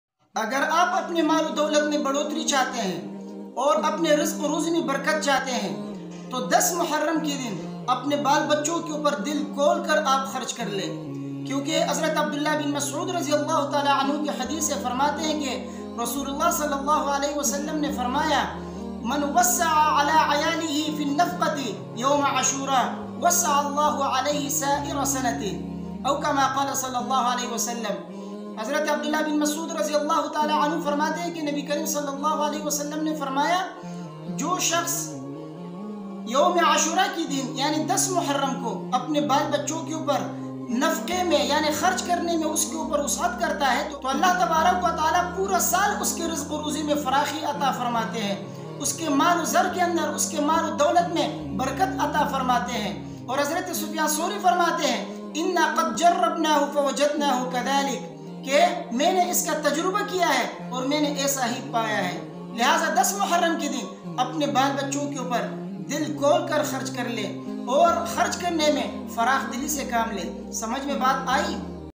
10 MUHARRAM ko Ghar Walo Par Kharch Karne Ki Fazilat | Muharram 2023
10 MUHARRAM ko Ghar Walo Par Kharch Karne Ki Fazilat | Muharram 2023

is video me ye bataya giya hai k 10 moharram ko Apne biwi Bachon par kharch Karne Ki kitni Fazilat hai aap ke dolat Aapke maal Apke rizq me barakt or izafa Hoga hadees me bayan Kya hai kharch Karne se rizq me barakt or farakhi hoti hai

AMEER Hone Ka WAZIFA | 10 MUHARRAM ka Powerful Wazifa | 10 Muharram Mein Ye 2 Kaam Zaroor Karen